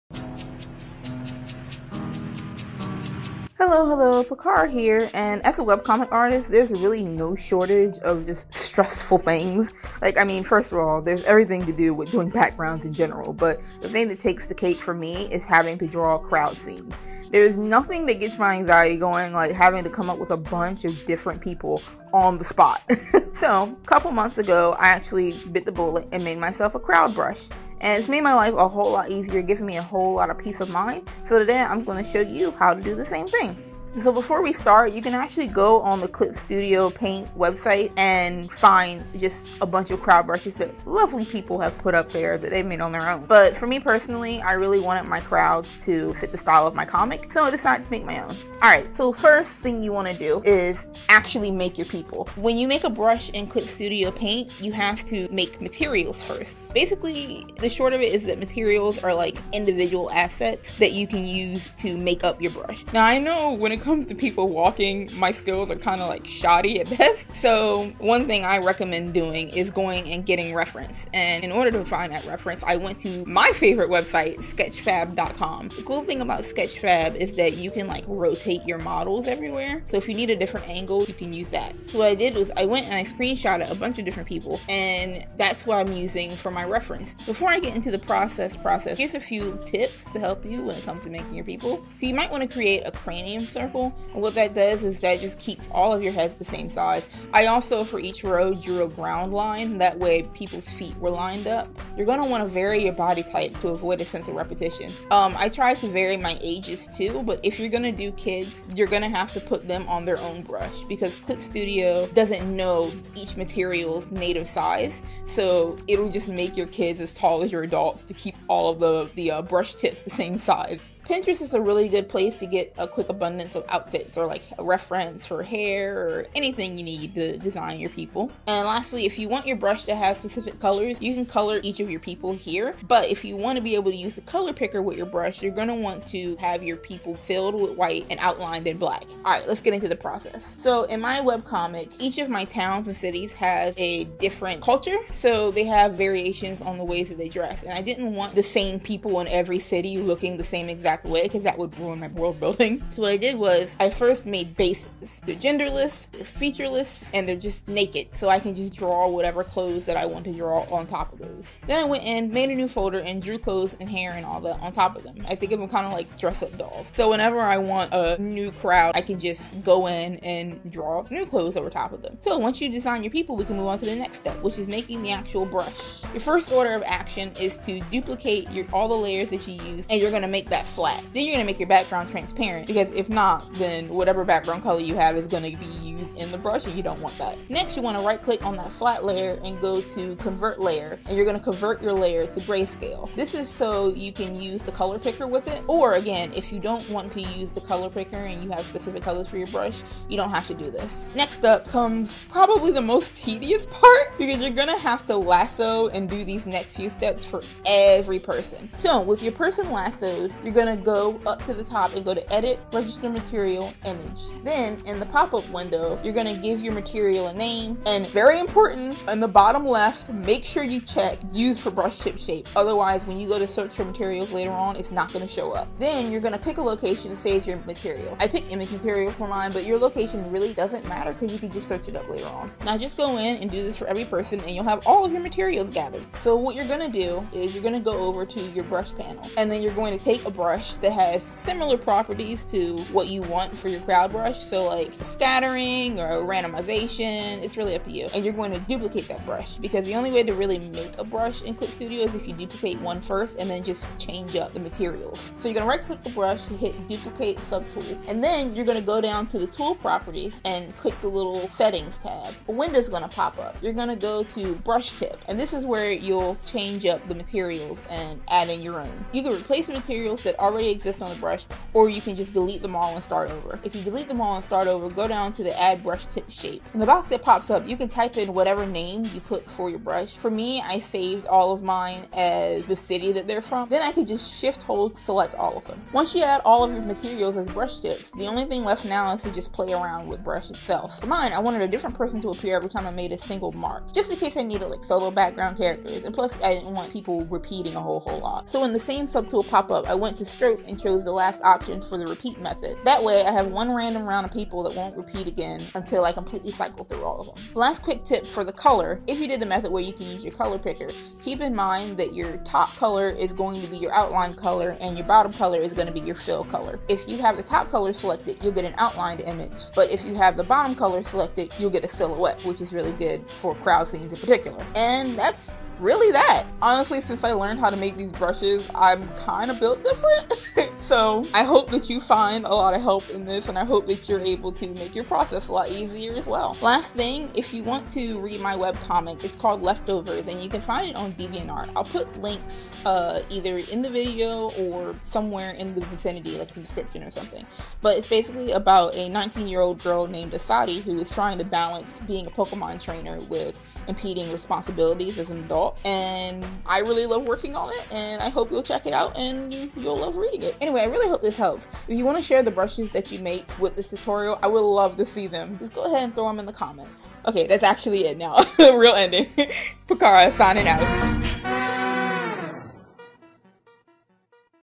Make a Crowd Brush in Clip Studio Paint!
Don't mind me, just making the video I wish I had when I was trying to make my own brush! Hope this helps!!